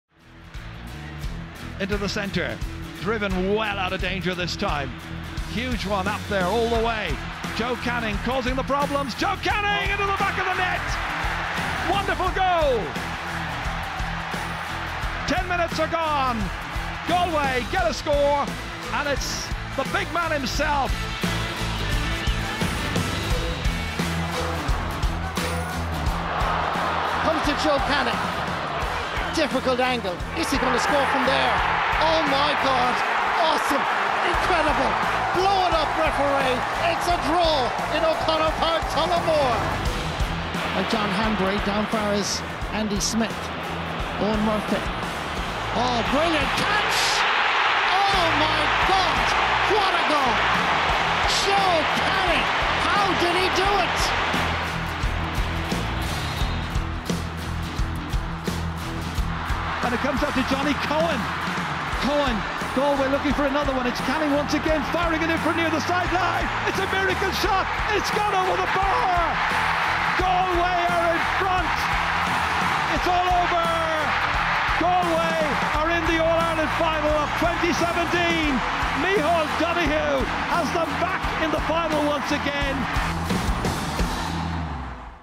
GAANOW: 'The Joe Show'
One of the stars of Galway Hurling! GAANOW looks back at some of the great moments in the career to date of Joe Canning - look back at some of the Portumna man's great points & goals for the Tribesmen